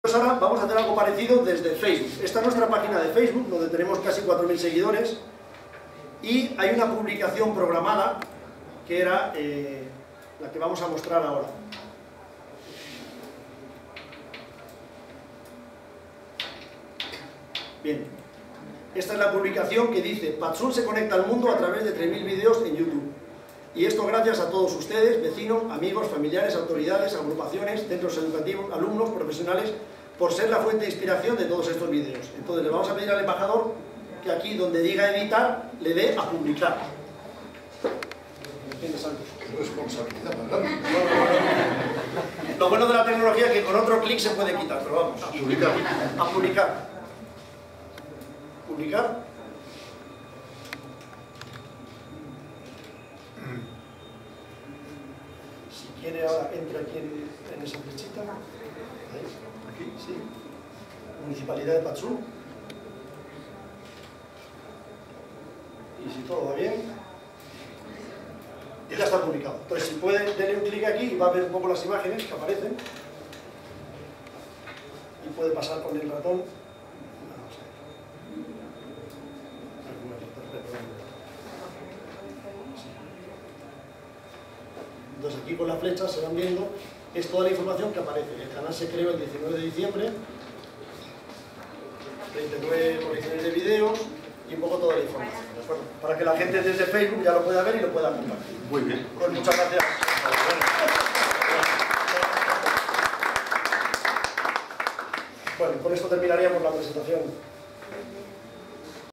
Lanzamiento mundial por el Embajador de España
Patzún, Lanzamiento mundial de 3,000 videos en YouTube por el Embajador de España durante la visita del Embajador de España en Guatemala, Alfonso Portabales, la Ministra de Trabajo Aura Leticia Teleguario y la Vice Ministra de Prevención Social Griselda González Navichoc.